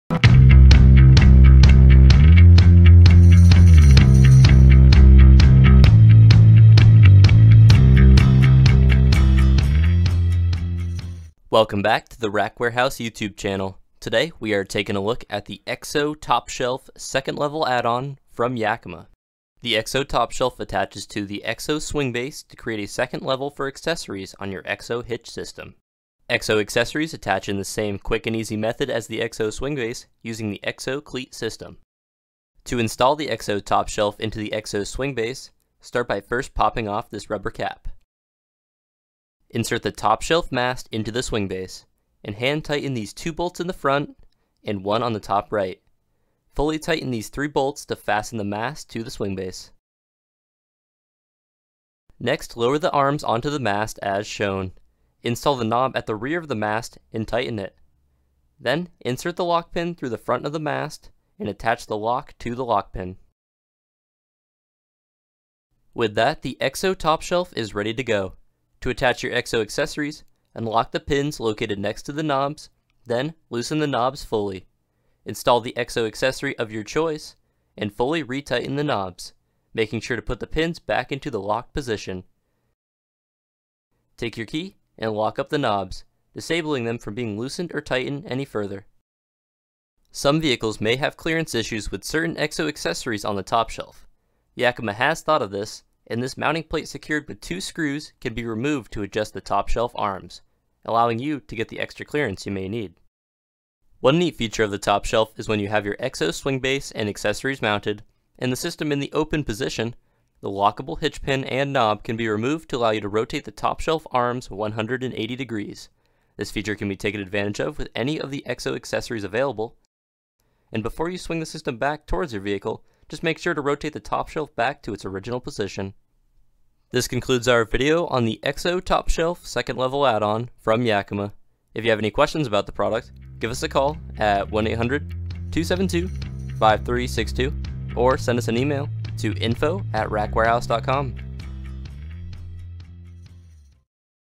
Yakima 8002716 EXO TopShelf Second Level EXO System Base
Yakima 8002716 EXO TopShelf Second Level EXO System Base - Installation and Product Overview.

The Yakima 8002716 EXO TopShelf Second Level EXO Hitch accessory turns the Yakima EXO SwingBase System into one of the best hitch cargo management systems on the market today. The EXO TopShelf is only one of many accessory attachments available for the Yakima EXO SwingBase unit. The EXO TopShelf 8002716 Second Level doubles your hitch capacity with a second tier of super accessible storage. Whether you're cycling and camping, skiing and hiking or simply needing to double your load, the EXO TopShelf can mix-and-match EXO accessories to make any adventure easier.
Here are a few more Yakima EXO Accessory Attachments available for the EXO SwingBase Hitch System: EXO BackDeck, EXO DoubleUp, EXO GearLocker, EXO GearWarrior, EXO SnowBank, and EXO LitKit.